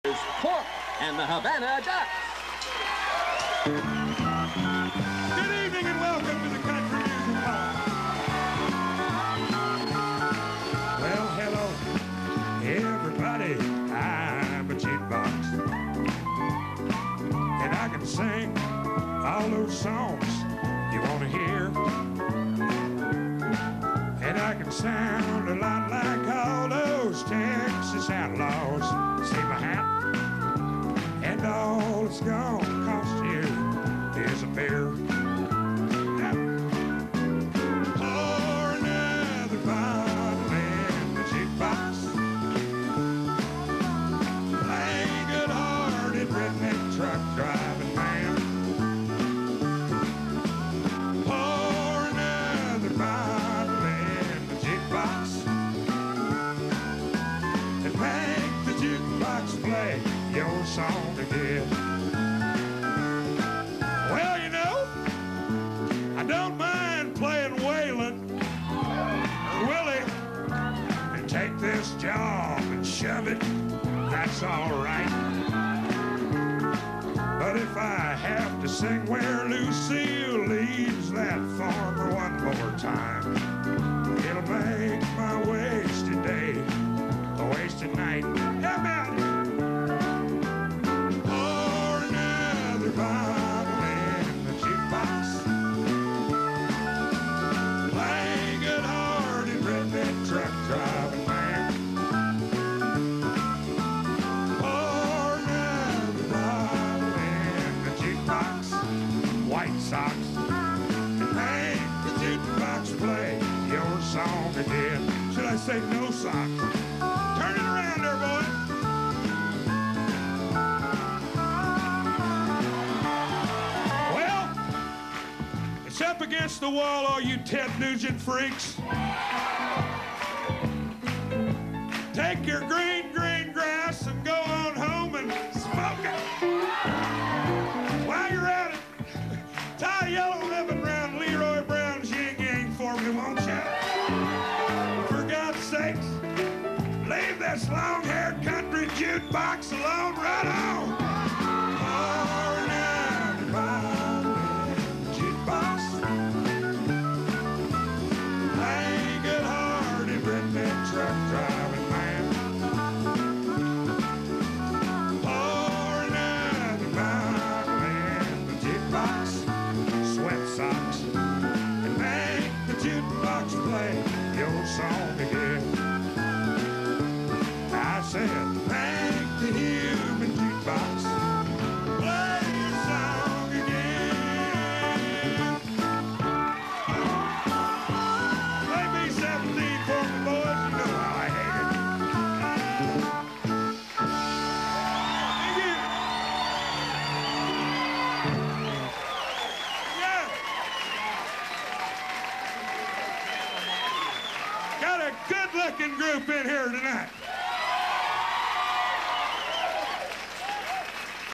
Pork and the Havana ducks1.mpg
Jerry {Pork } Armstrong we will miss you forever, I had the best times of my life watching you and the band at different places through the years and I really miss those days at Ds country lounge every weekend, thanks for the memories,you will always be missed by us all, good bye friend !